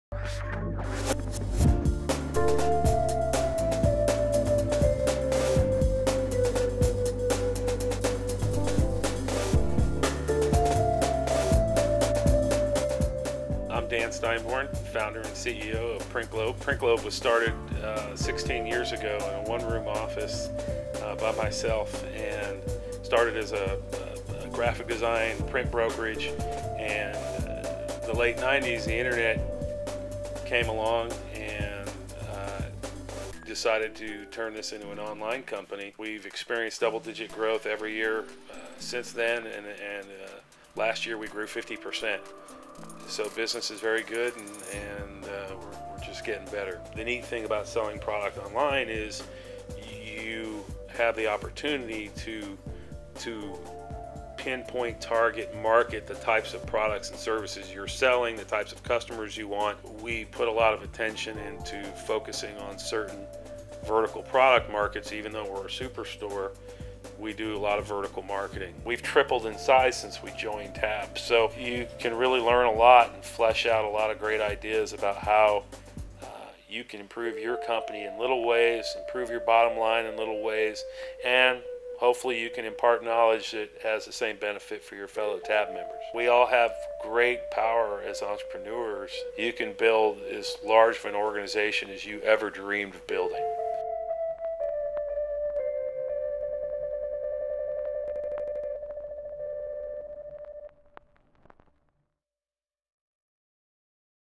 Dan Steinborn, PrintGlobe - Member of The Alternative Board of Austin
Dan Steinborn, CEO of PrintGlobe Inc., gives an overview of his printing company and how it benefits from The Alternative Board.